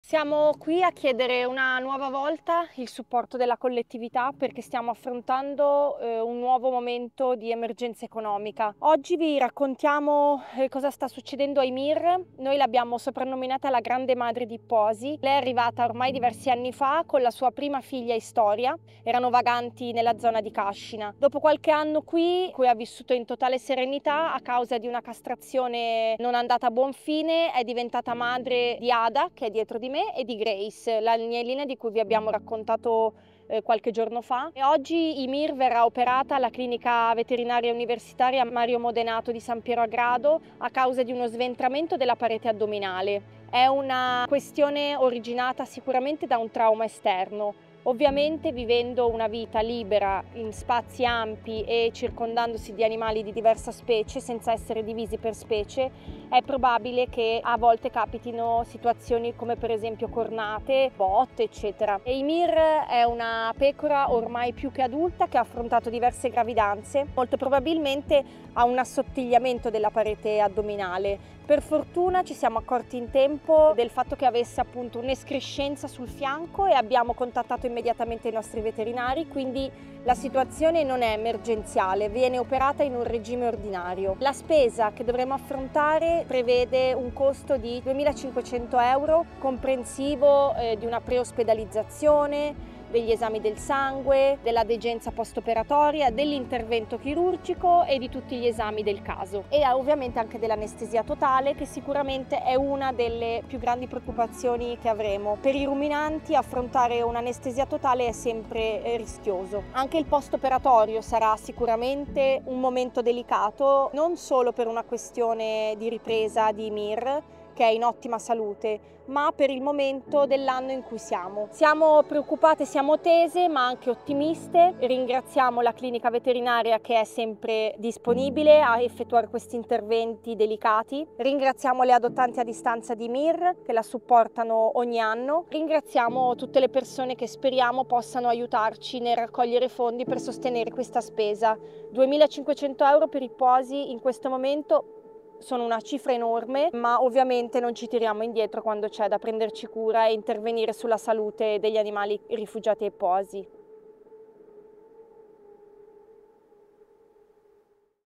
Ymir ha bisogno di noi!
Abbiamo lanciato una raccolta fondi su @eppela e vi chiediamo di condividerla e, se potete, di donare per aiutarci a saldare quanto prima la fattura alla clinica.

Ymir è stata operata per uno sventramento della parete addominale, causato probabilmente da una cornata o da un altro trauma esterno.
L’intervento è avvenuto all’Ospedale Didattico Veterinario universitario Mario Modenato a San Piero a Grado ed è andato molto bene: lei è in salute e con appetito, resterà qualche altro giorno in degenza e poi, se tutto sarà a posto, tornerà a casa sua, con la sua famiglia. ♥️

Il costo complessivo dell’operazione è di 2.500 euro, una cifra in questo momento enorme che però non ci rifiuteremmo mai di sostenere per supportare una delle soggettività di cui ci prendiamo cura.

Ymir e Ippoasi hanno bisogno di te e di noi! ❤️‍🔥